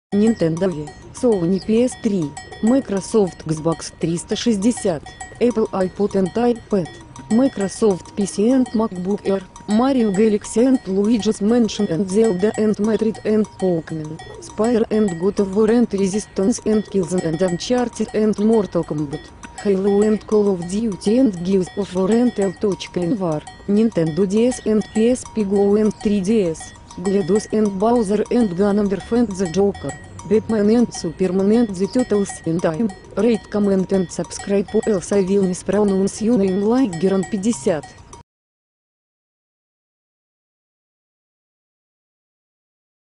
russian girl mispronounces video game words
she butchers your favorite titles